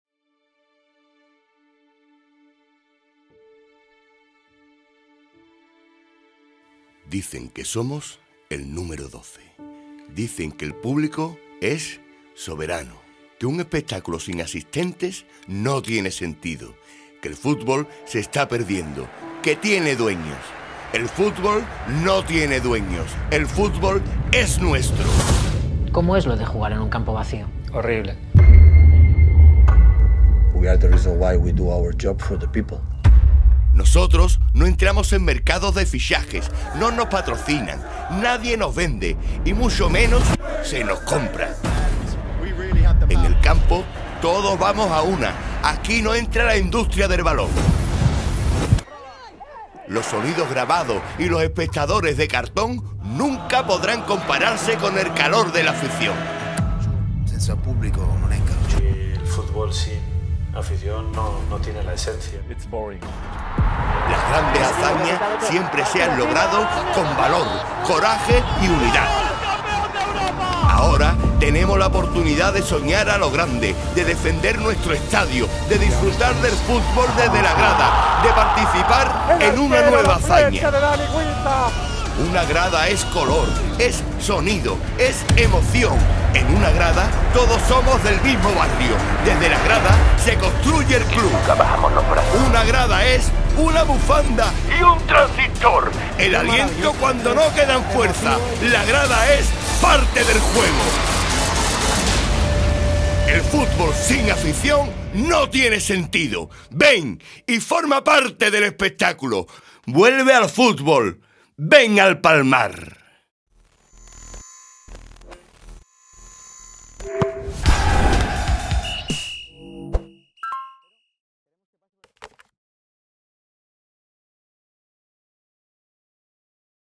Spot de la Campaña de Socios del Atlético Sanluqueño C. F. para la Temporada 21/22
Vídeo promocional de la Campaña de Socios del Atlético Sanluqueño C. F. para la temporada 2021/2022.

Esta campaña ha sido basada en la ausencia de la afición en las gradas y en la conmemoración de una de las hazañas más importantes de la historia, la primera vuelta al mundo. Esta hazaña partió y llego desde Sanlúcar de Barrameda, celebrándose ahora su 500 aniversario por lo que el club se abandera de la historia de su ciudad para llevar el nombre de Sanlúcar de Barrameda por todo el territorio nacional.